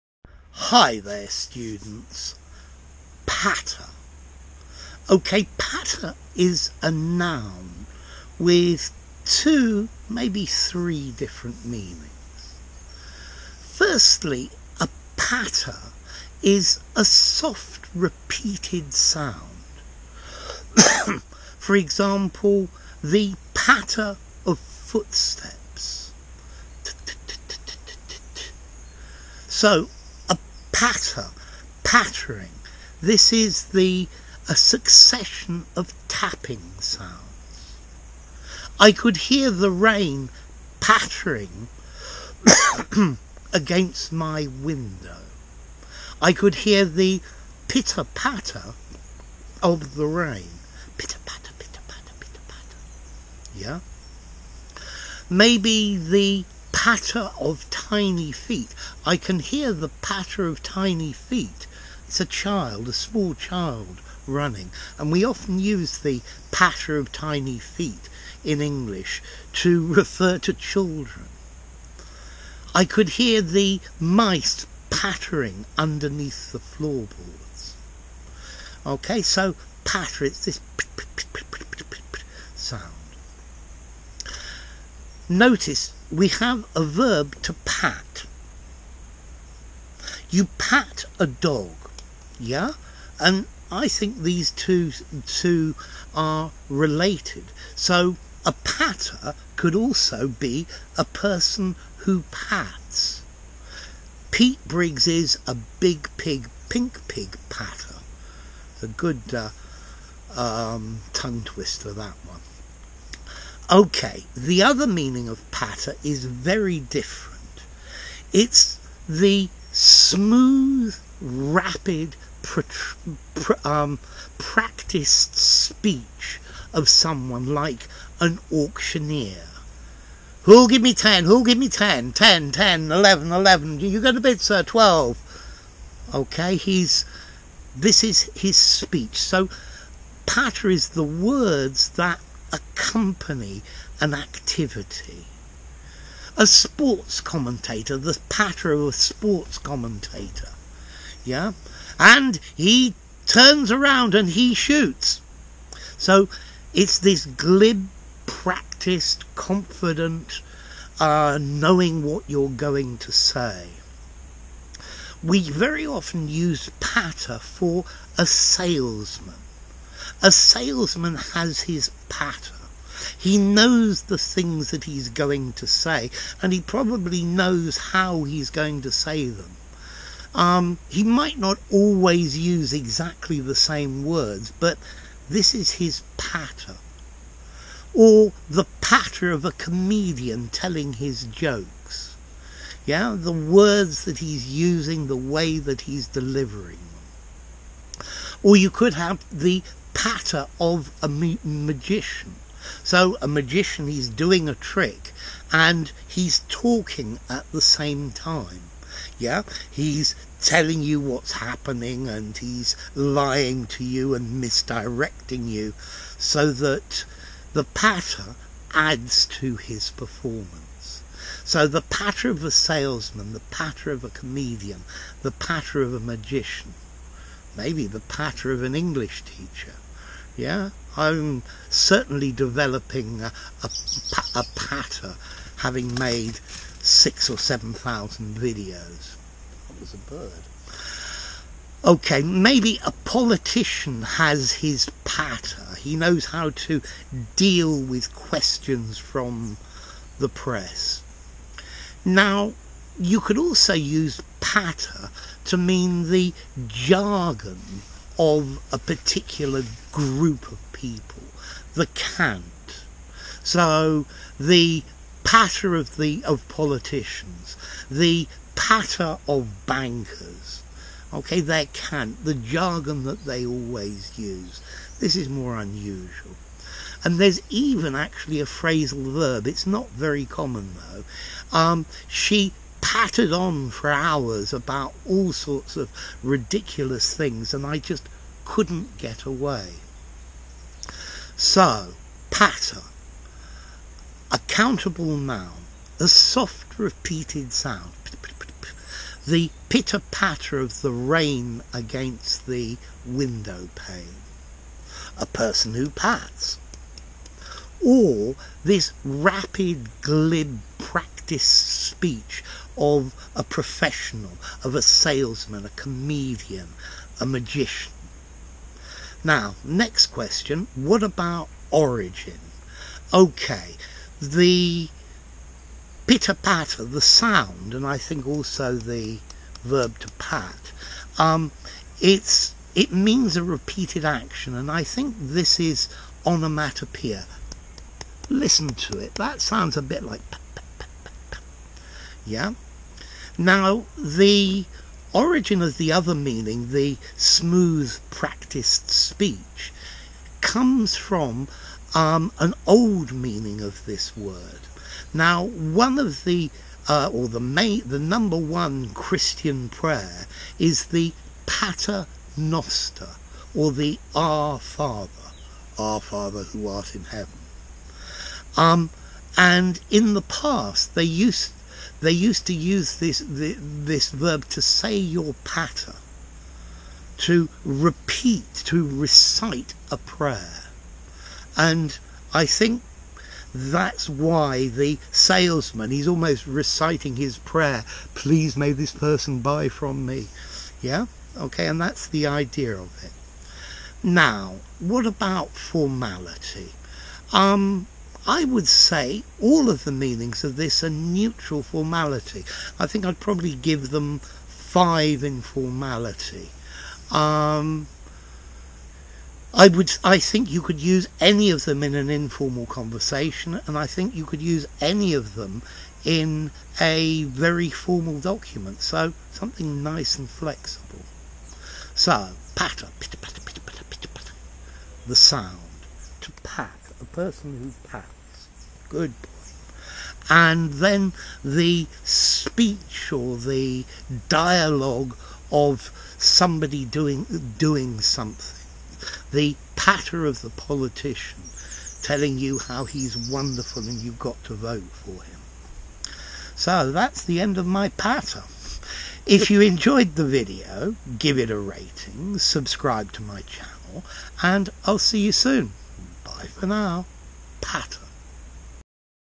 🔵 Patter - Patter Meaning - Pitter-Patter Examples - Patter Defined
What does patter mean? What is patter? What is the meaning of patter? A definition of patter. The video gives various example sentences of patter in context for you to be able to use patter correctly when speaking and writing.